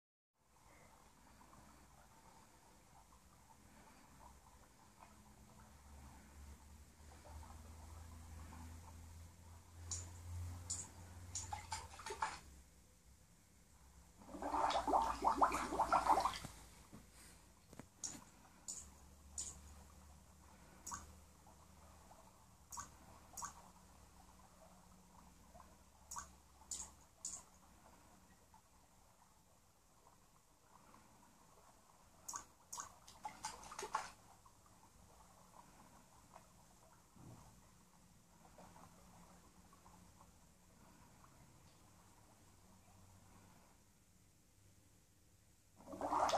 Funny cat catch fish on iPad . Cats playing games. Super cute kitten
Funny cat playing ipad